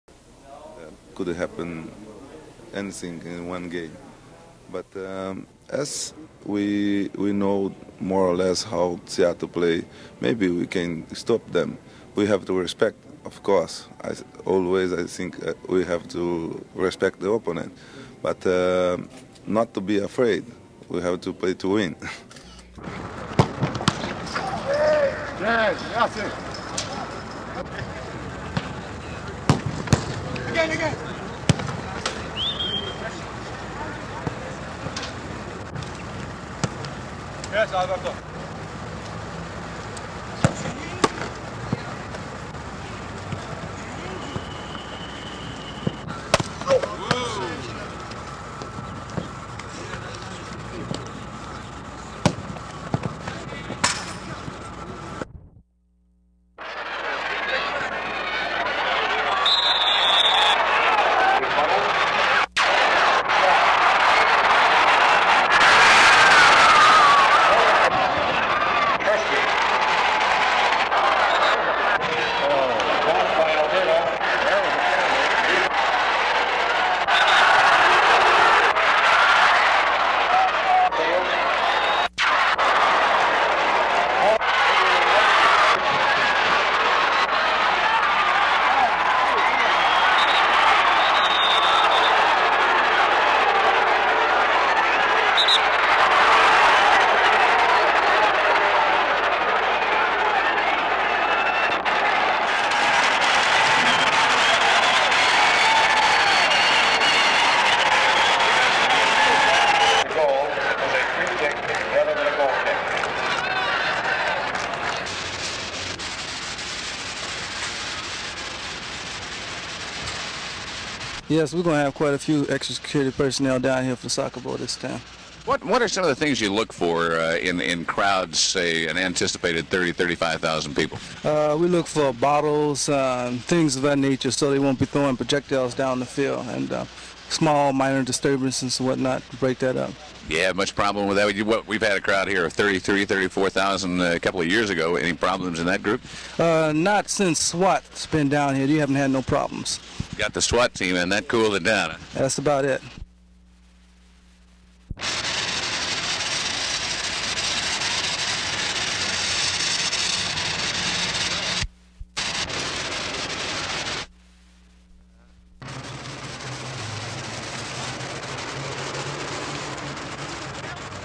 Pelé plays last competitive game Portland - August 1977 | KATU In The Archives
Iconic soccer player Pelé played his final competitive game in Portland in August of 1977, in the New York Cosmos v. Seattle Sounders championship match hosted at Civic Stadium (now Providence Park).